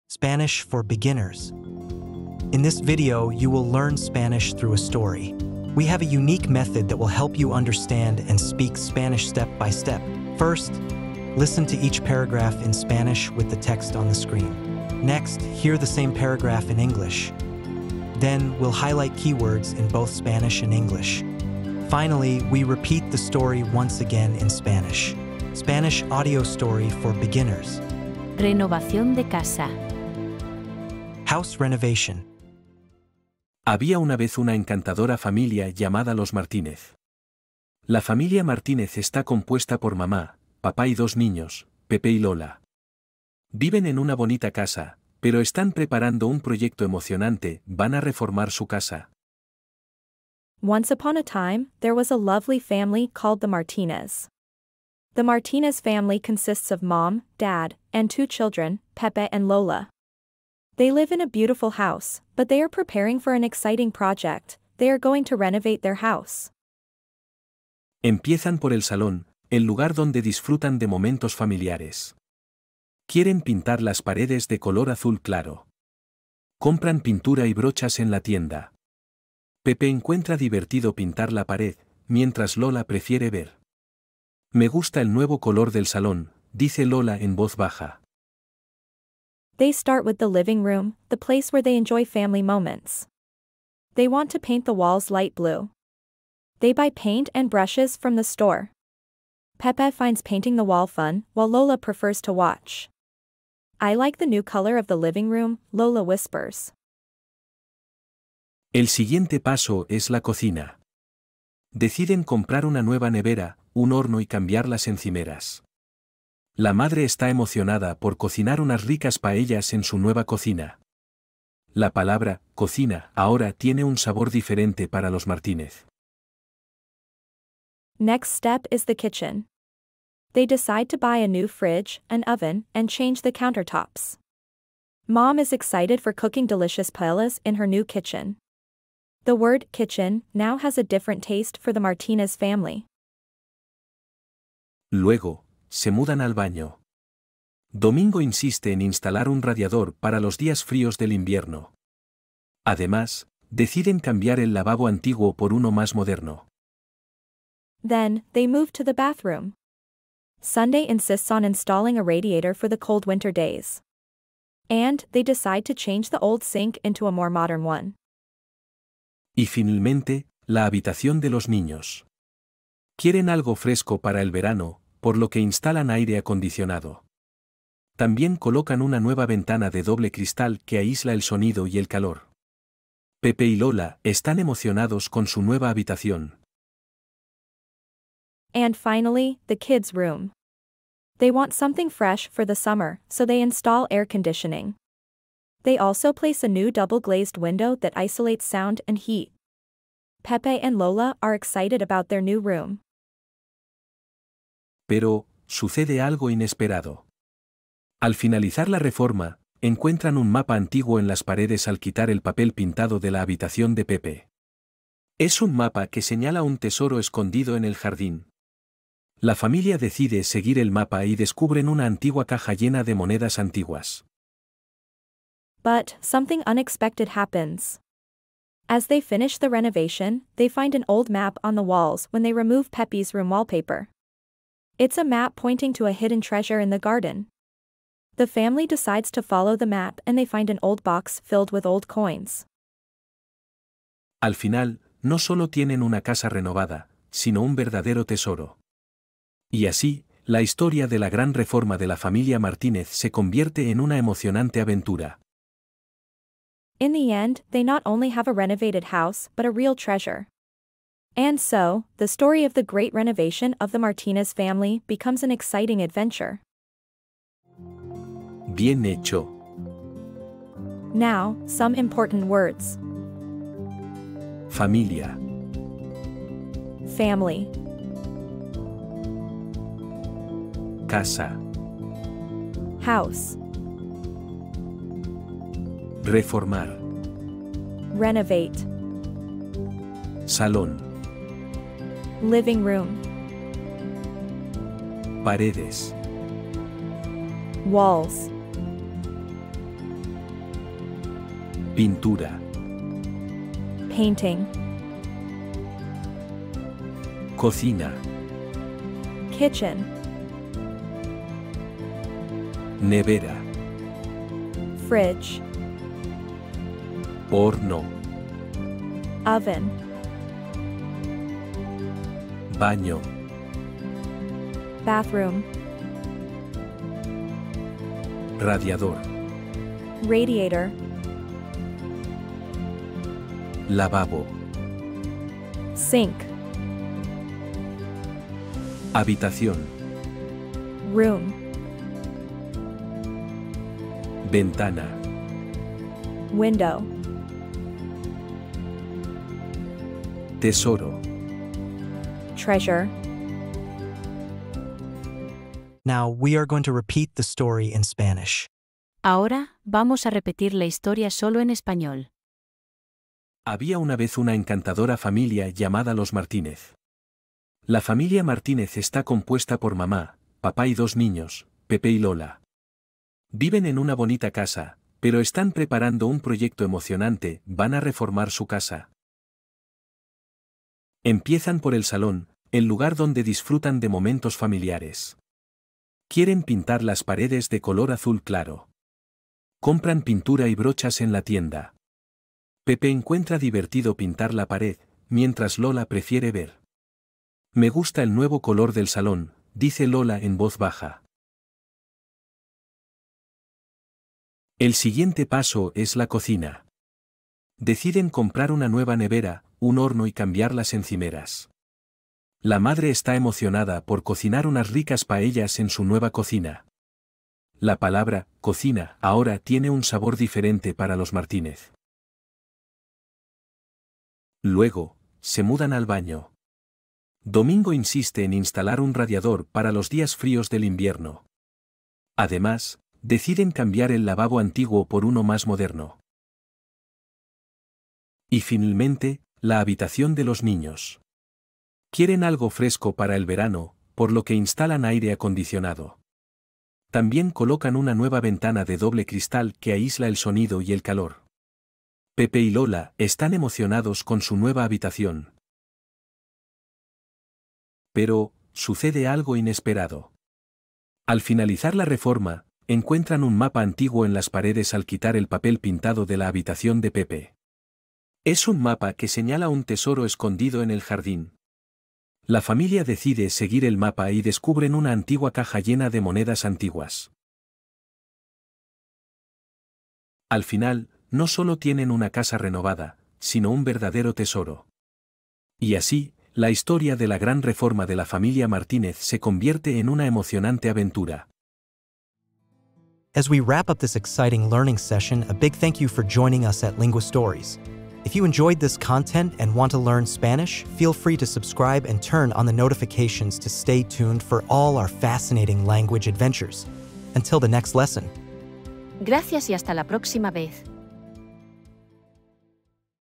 START LEARNING SPANISH with a Simple Story (House Renovation)
Join the Martínez family as they embark on an exciting home renovation project in this easy Spanish audio story, perfect for beginners! 🇪🇸🔨

00:00 Introduction and explanation of learning Spanish through stories
00:25 Story in Spanish and English
04:28 Vocabulary from the story
06:00 Story in Spanish only

In "Renovación de Casa," you'll follow the Martínez family through the ups and downs of renovating their home. This engaging story is packed with essential Spanish vocabulary and phrases related to home improvement, making it ideal for beginners looking to enhance their language skills.

📖 Story Highlights:

Learn key Spanish words and phrases about home renovation.
Improve your listening skills with clear, slow-paced narration.
Reinforce your understanding with an English recap at the end.
Whether you're just starting out or looking to practice your skills, this story will help you immerse yourself in the Spanish language.